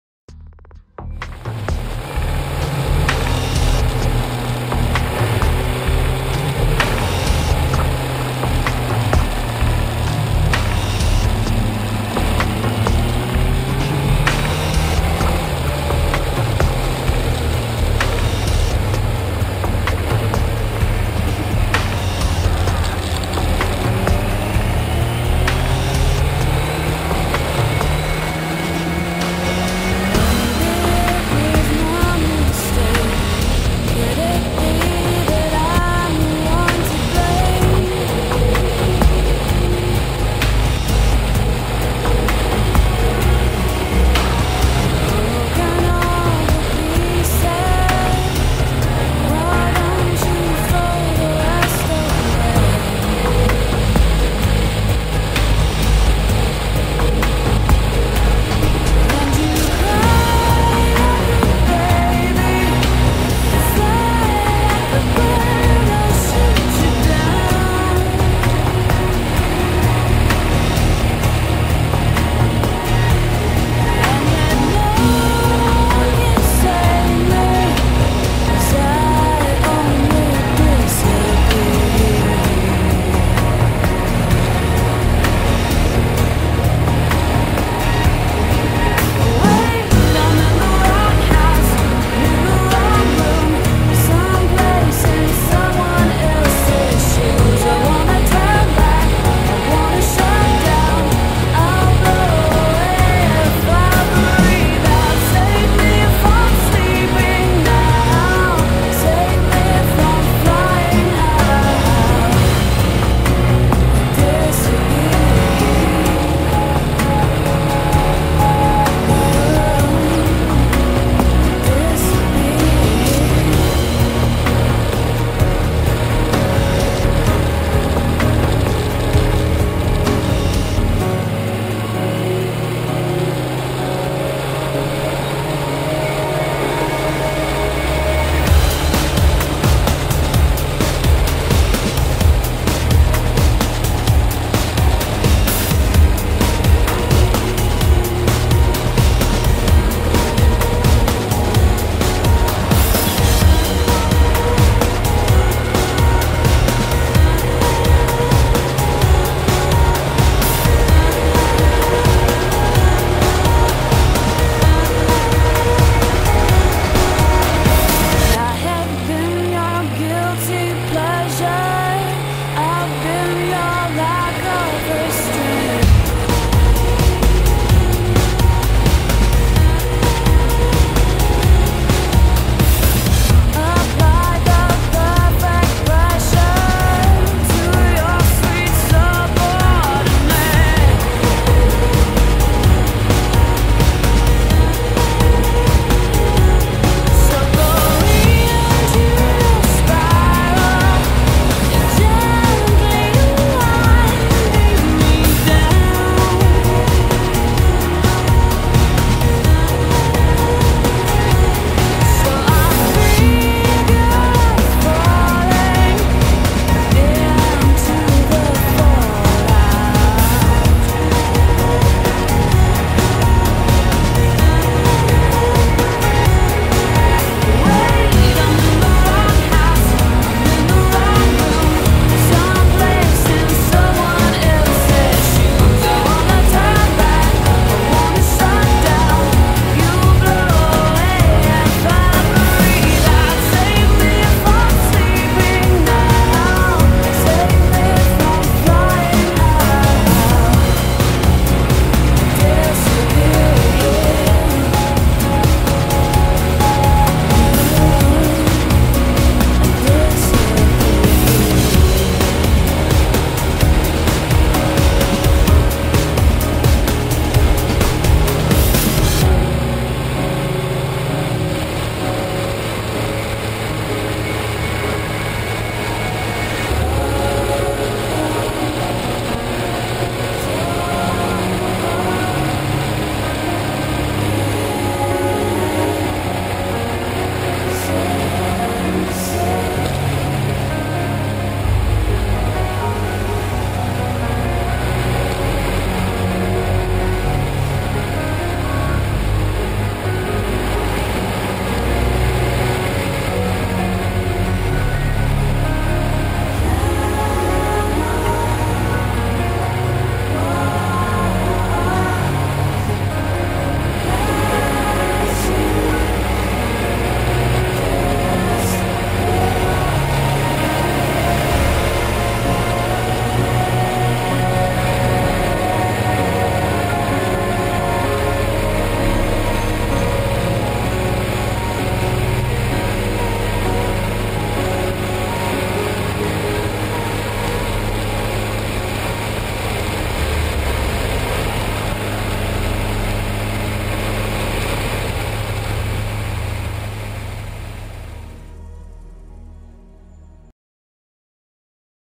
Snaking through the hills
out in the hills on the bike with a GoPro